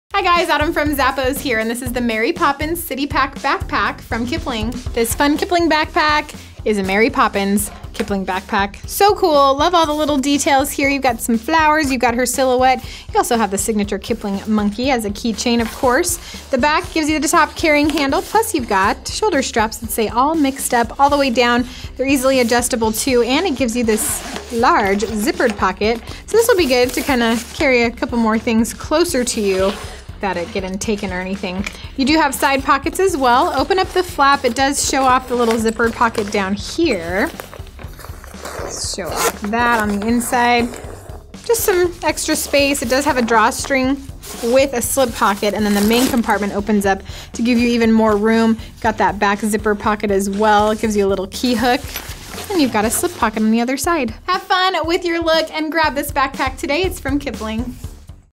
Kipling Mary Poppins Citypack Backpack SKU: 9188776
Don't stick to any rule have fun with your style and throw on the Kipling&reg; Mary Poppins Citypack Backpack.
Made of man-made material with perforated detail.
Flap with magnetic snap and zipper closure.
Top carrying handle with dual adjustable shoulder straps.
Comes with signature Kipling monkey key chain.
Exterior front zip pocket.
Three exterior slip pockets.
Exterior back zip pocket.
Signature logo embossed detail at front.
Flat bottom.
Lined interior.
Interior zip pocket.
Three interior slip pockets.
Imported.